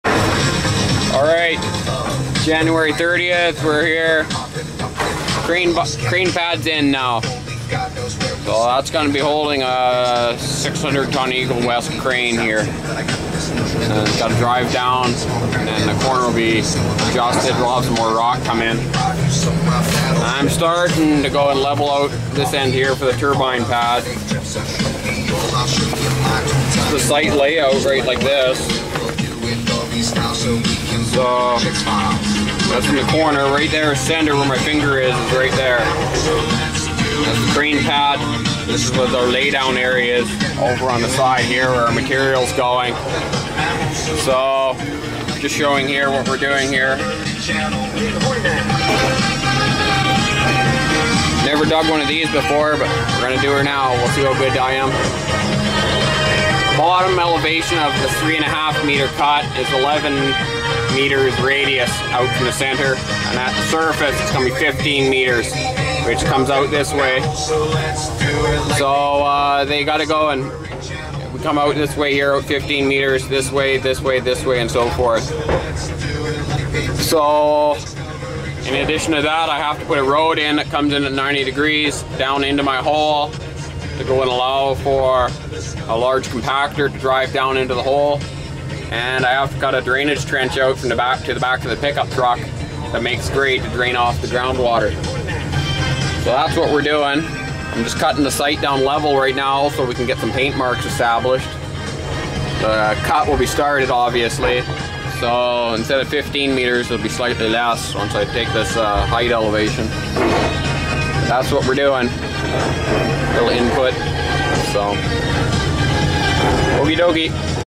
Constructing a wind turbine location on North Vancouver Island
January 30, 2013 North of Nahwitti Lake, North Vancouver Island, BC. Cape Scott Windfarm Construction. This shows the just-completed crane pad for the large 600 ton crane from Eagle West that will be standing the turbine tower in this location. I showing the location drawings for turbine no. 36. This will be a 3.5m cut that will be cut in a circle and have a road access for a roller compactor and be drained. It will be 11m radiusing out from the shown legal centre stake at bottom. First time for me doing a job like this. Showing how a swamp is transformed into a multi-million dollar wind turbine location. I am in a Kobelco SK350LC-9 excavator.